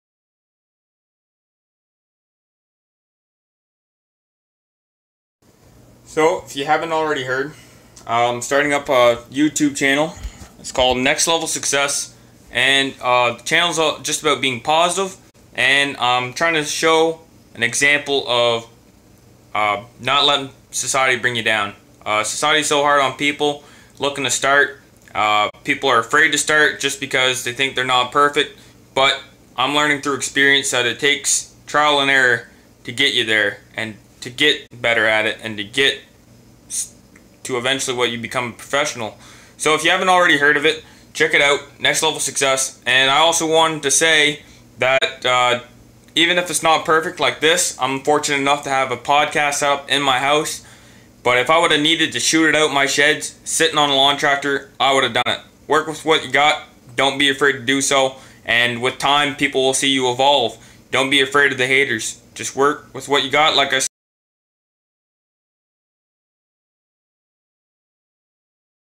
Don't be afraid to start, and work with what you got!
My set up is far from perfect, although i am fortunate enough to have a podcast set up like i do. It is okay not to be perfect as long as you start! The perfection will come in time through trial, and error.

I try to stay as active as i can be with my busy schedule, but if you want to see more in my slow, but growing social media platforms check me out on 👇

Or my youtube channel
NEXT LEVEL SUCCESS